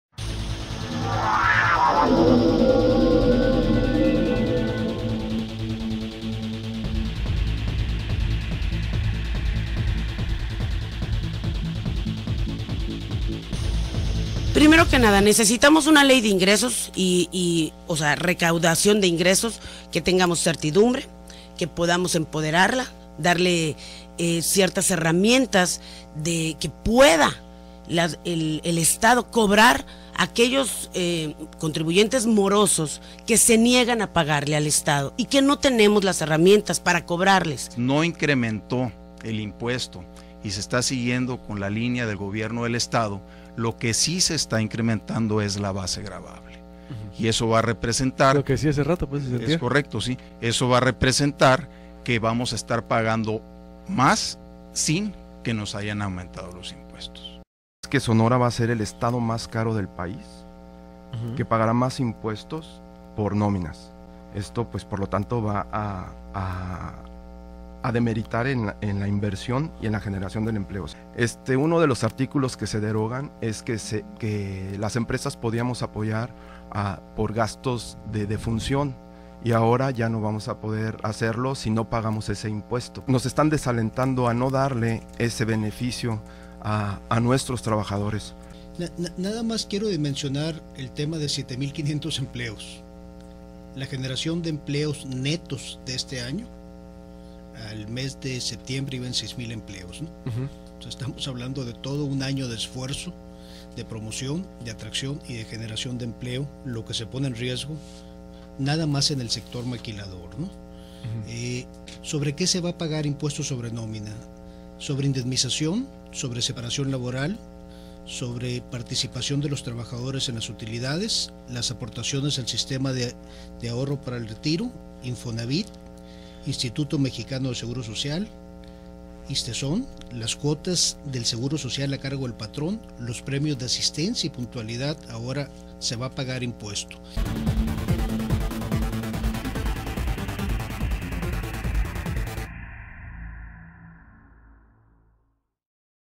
Cuestionan empresarios falta de transparencia y tiempo para revisar Paquete Fiscal
Los líderes empresariales Marcelo Meochi, Óscar Serrato y Gerardo Vázquez cuestionaron la premura y falta de transparencia en la aprobación del Paquete Fiscal 2017 por parte de los diputados del Congreso del Estado de Sonora.

“Los diputados son como un huracán, que en una vorágine legislativa son tan capaces en 3 horas de aprobar 72 leyes de ingresos, los ingresos y egresos del Estado, reformar la Ley de Hacienda y el Código Fiscal, y no sabemos qué sorpresas más”, aseveró Óscar Serrato.